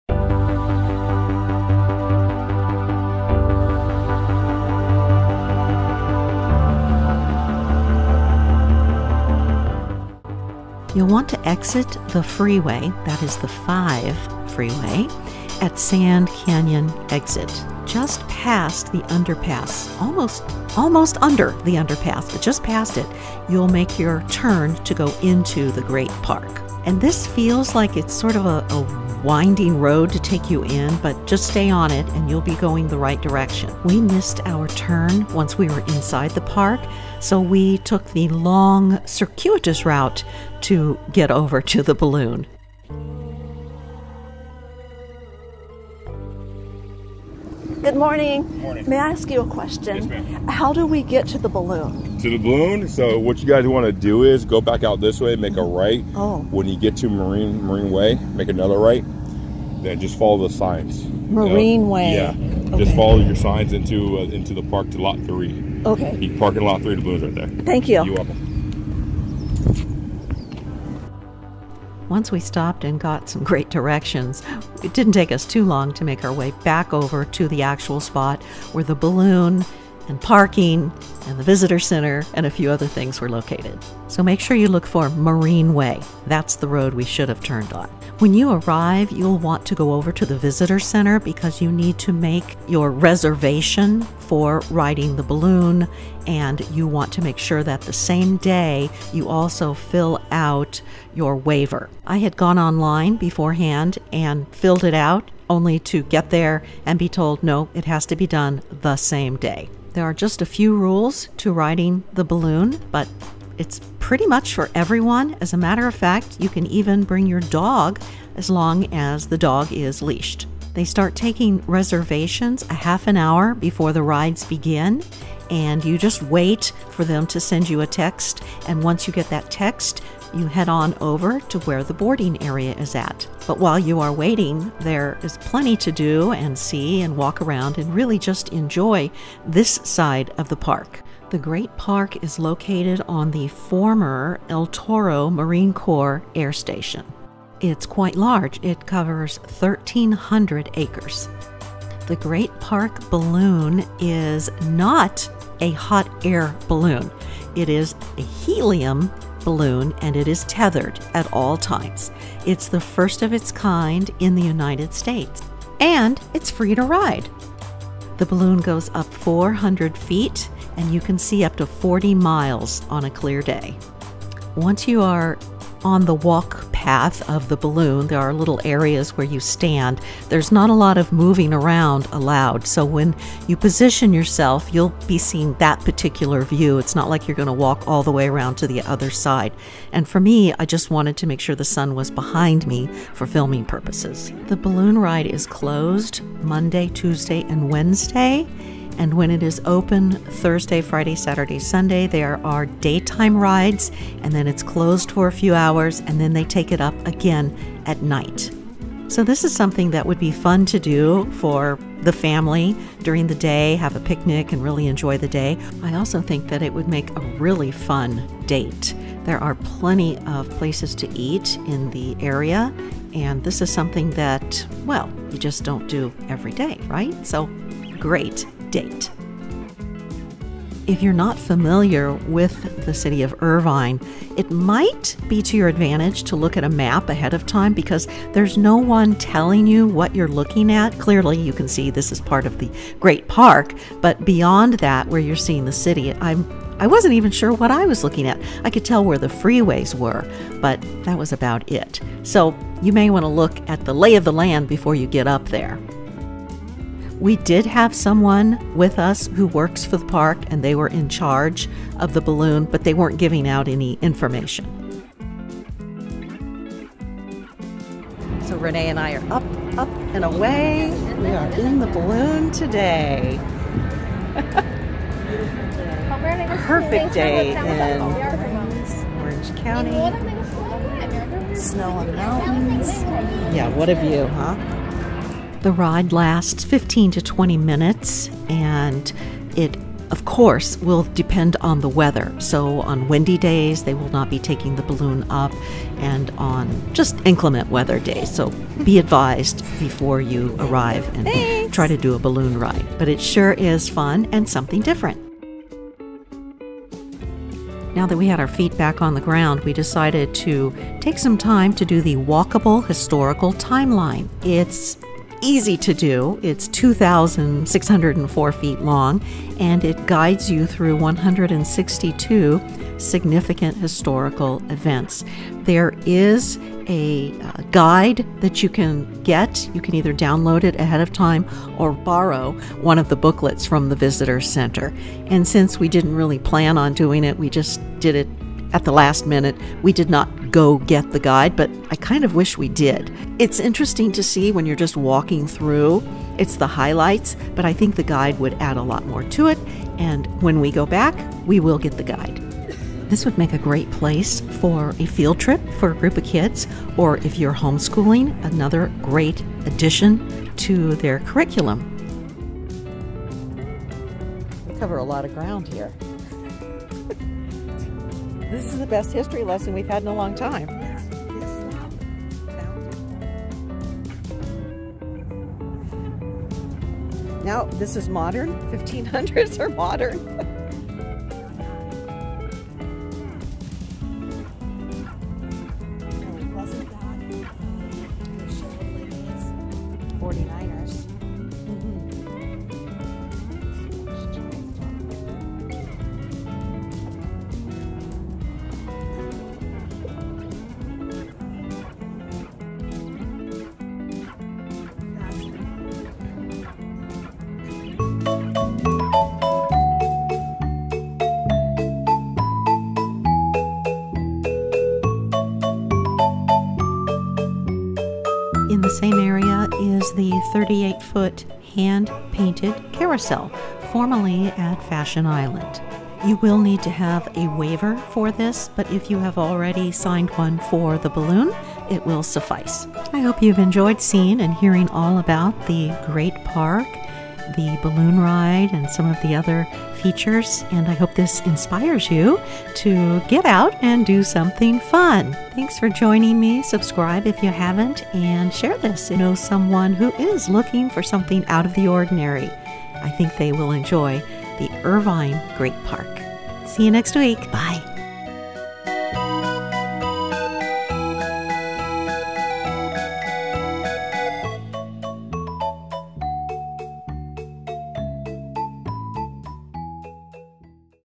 IRVINE GREAT PARK; Balloon & Carousel rides & so much more!
Irvine Great Park offers plenty of activities for families, individuals, children, and outdoor enthusiasts. From athletic fields and walking trails, to playgrounds and a carousel, there is something for everyone.
Renee and I came specifically for the balloon ride, but discovered so much more!

Great Park
8000 Great Park Blvd.

I'm not sponsored or compensated; I just want to share fun things that I do, see, or have that I think others will enjoy or be interested in!